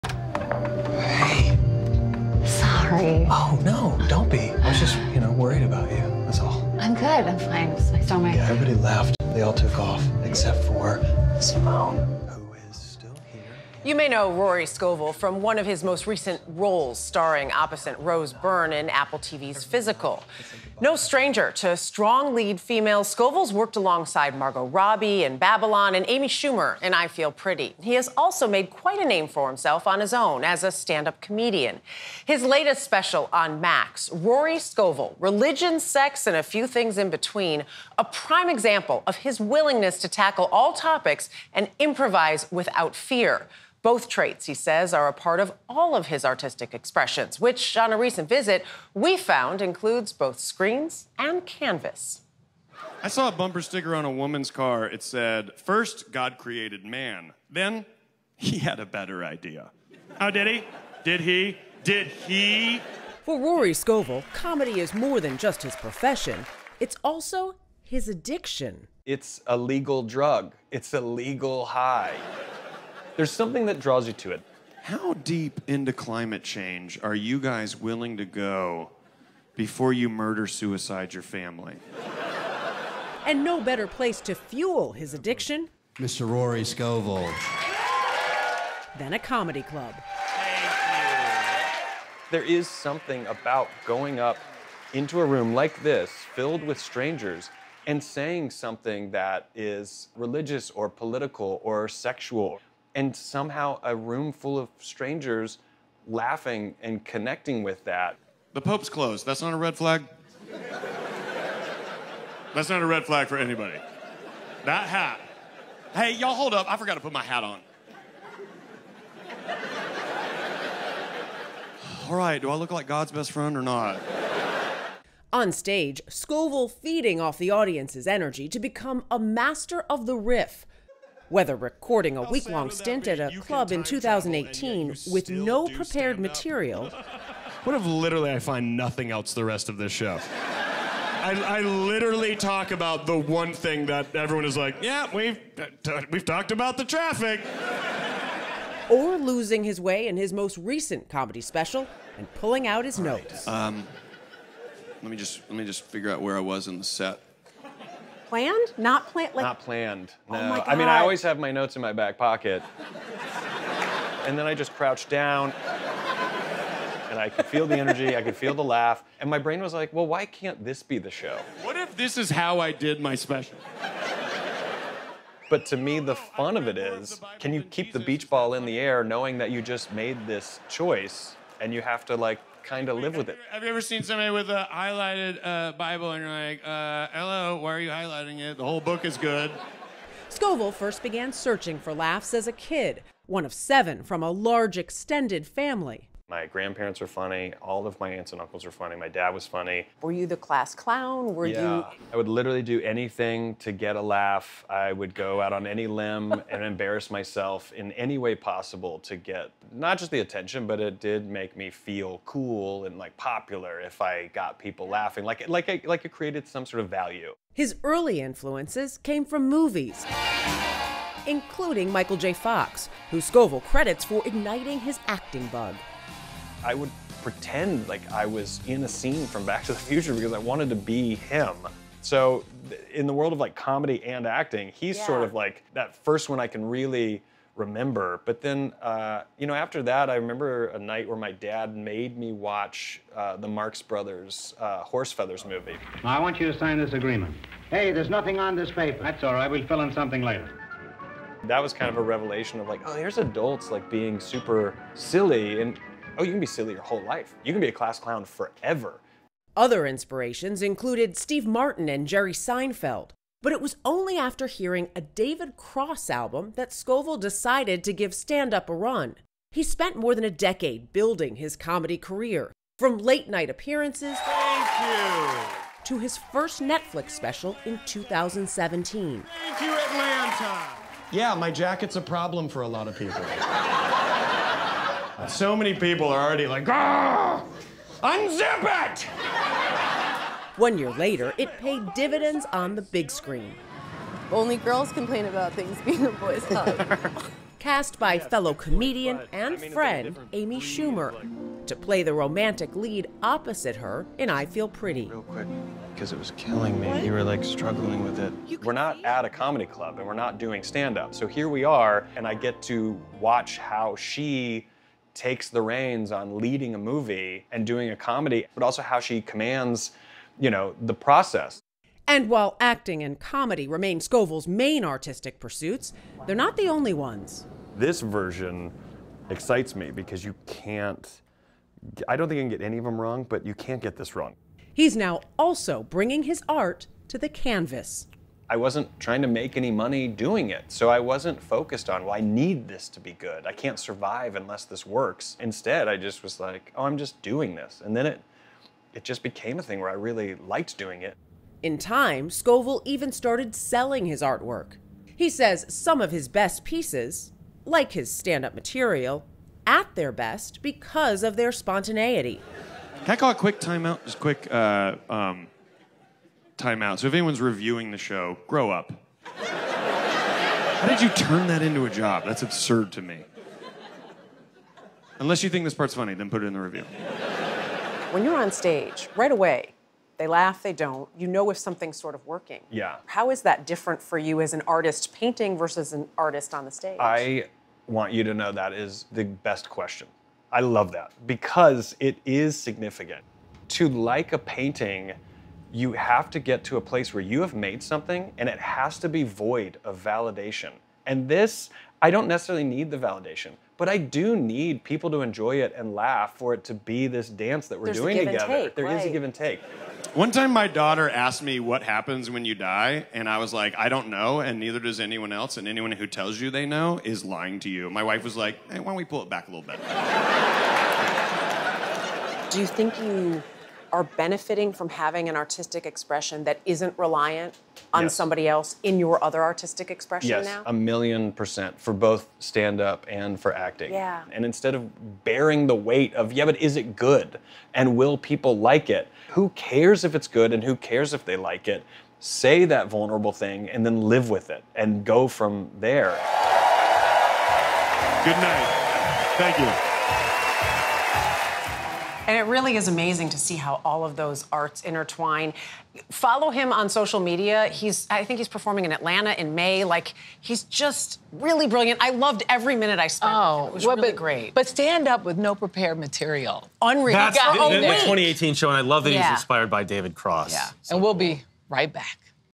Actor Rory Scovel on his long career and new comedy special
You may know Rory Scovel from his TV and film roles, but the actor's artistic vision is also visible on canvas and in his new comedy special on Max. Dana Jacobson sits down with the actor to discuss his life and career.